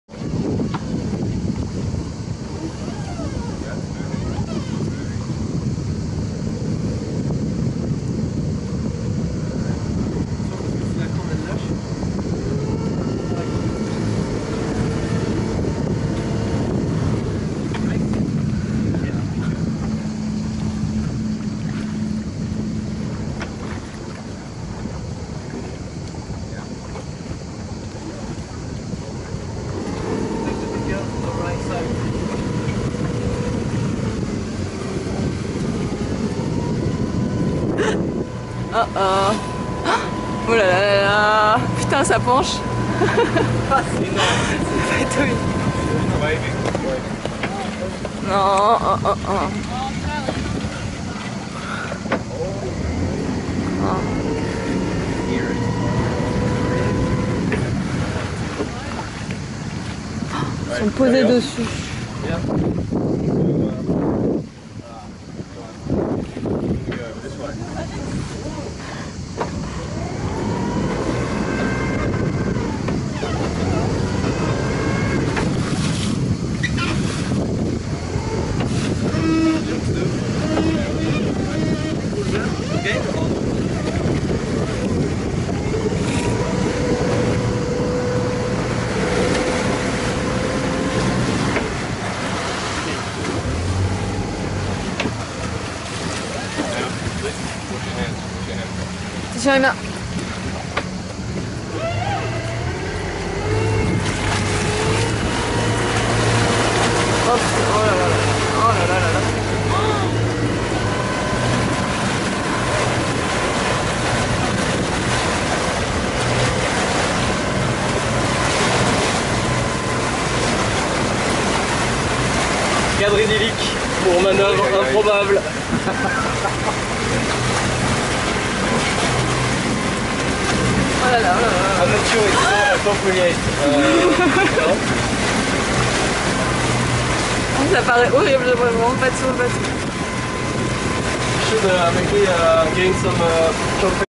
Rescuing shipwrecked Italians off the Skorpios Islands reef in Greece
Dragging some stranded sailers off a reef in Greece.